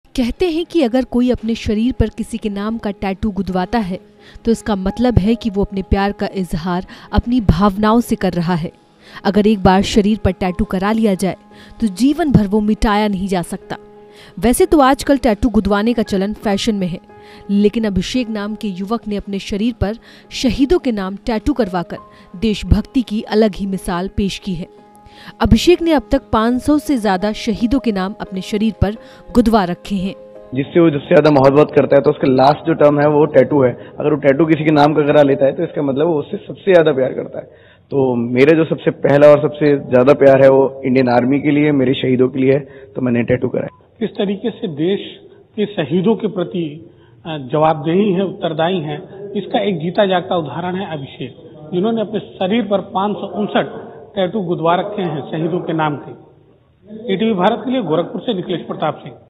अनोखा देश प्रेम : शरीर पर गुदवाया 500 से ज्यादा शहीदों के नाम
गोरखपुर : टैटू गुदवाने का चलन आजकल फैशन में हैं, लेकिन अभिषेक नाम के युवक ने अपने शरीर पर शहीदों के नाम टैटू करवाकर देशभक्ति की अलग ही मिसाल पेश की है. अभिषेक ने अब तक 500 से ज्यादा शहीदों के नाम अपने शरीर पर गुदवा रखे हैं.